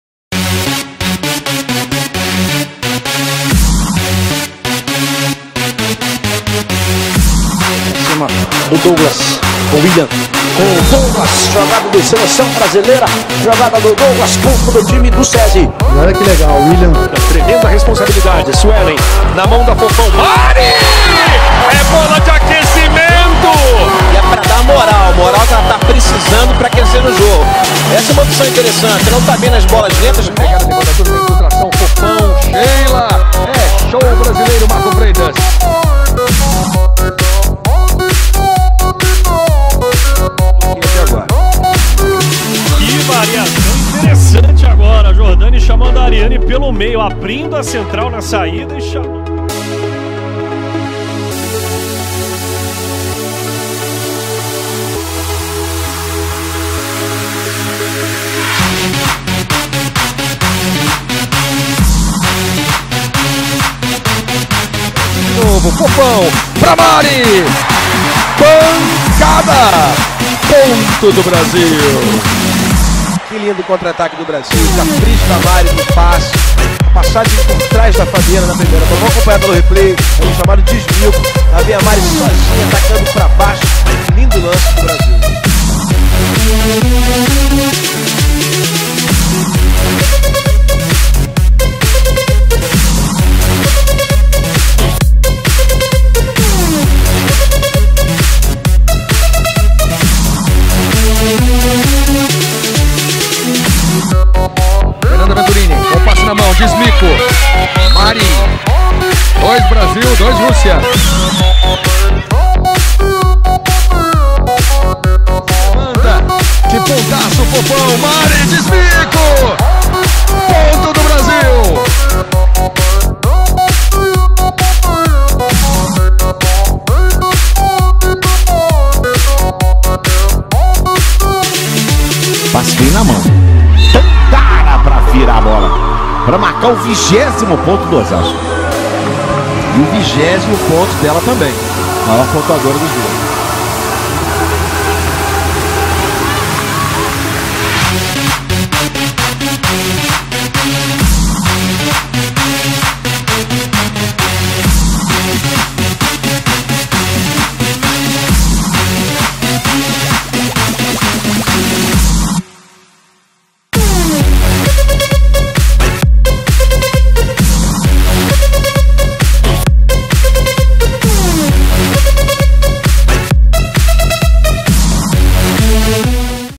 10 Desmicos do Vôlei Brasileiro
Um dos melhores golpes do vôlei, o desmico já foi muito utilizado pelos nossos jogadores! Separamos 10 desmicos dos nossos jogadores brasileiros! Enjoy it!